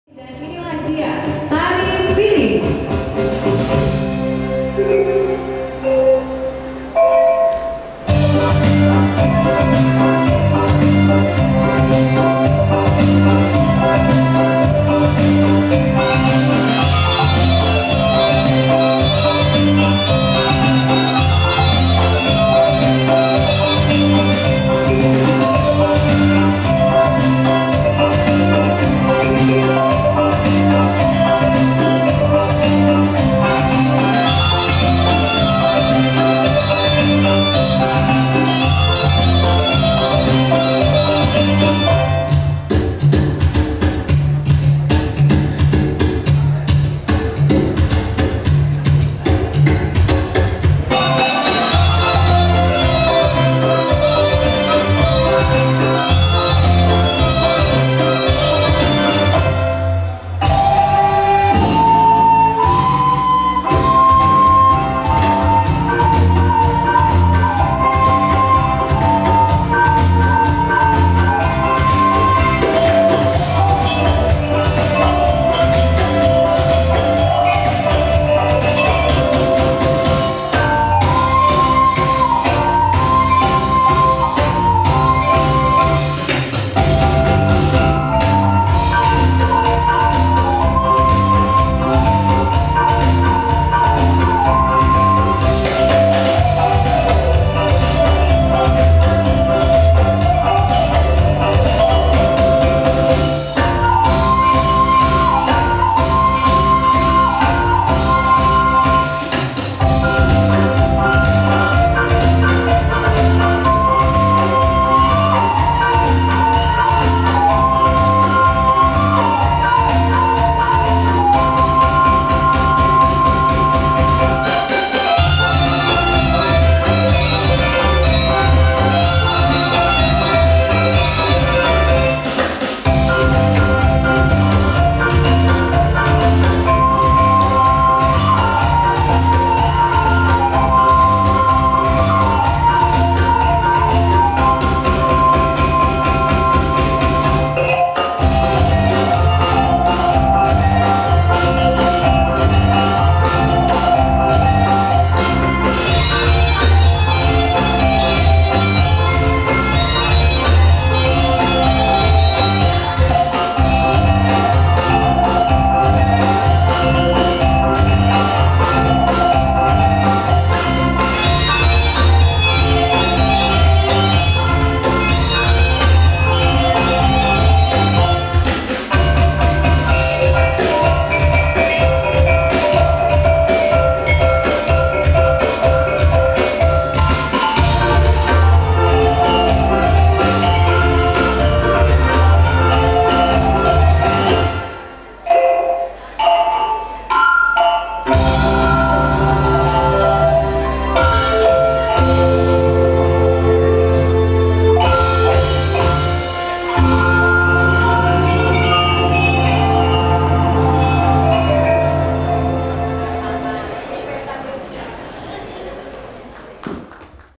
TARI PIRING | TARI ADAT SUMATERA BARAT
Tari Piring (Tari Adat Sumater Barat)
Wedding RAMA & DIRA - Gedung Serba Guna SUKA RIA - Palembang